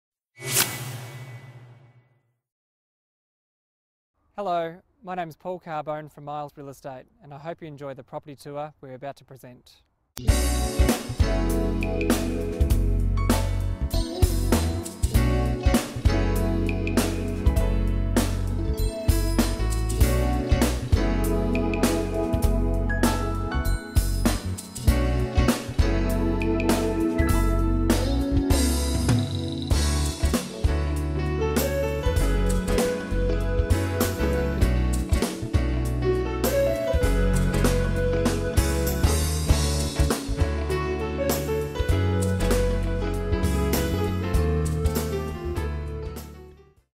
Miles Real Estate - 2/145 Yallambie Road - Paul Carbone
2/145 Yallambie Road, Macleod - Freestanding with wide garden frontage, bushland views and huge north-facing backyard, this brilliantly appointed single-level home stands alone as a superb surprise package on a 772m2 allotment (approx). Return driveway and carport add considerable convenience to a unique lifestyle opportunity with an emphasis on low-maintenance living, exceptional outdoor entertaining and proximity to amenities including shops, parks and primary school just footsteps away. Features three bedrooms, central heating, evaporative cooling, alarm system, security shutters, ducted vacuum, generous living room with ceiling fan, large casual dining area, light-filled kitchen, central bathroom, separate laundry, fabulous covered alfresco deck with bistro blinds and carport.
Miles Real Estate.

Paul Carbone : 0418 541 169 -